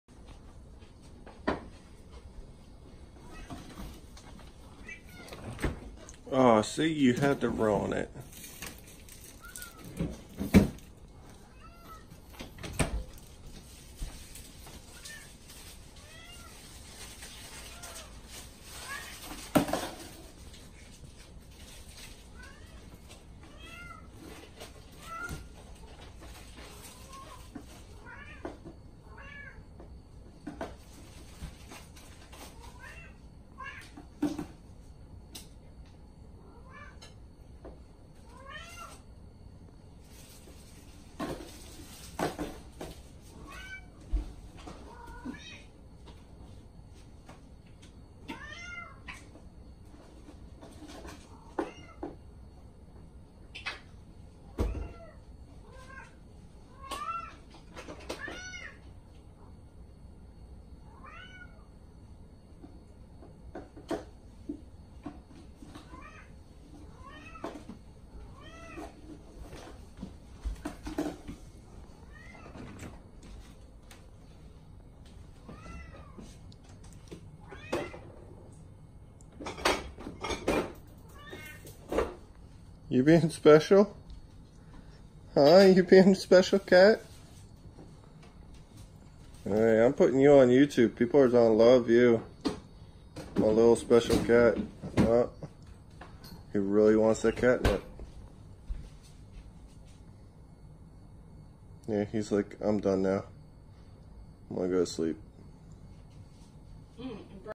Cat going CRAZY over catnip box
Black cats are the best!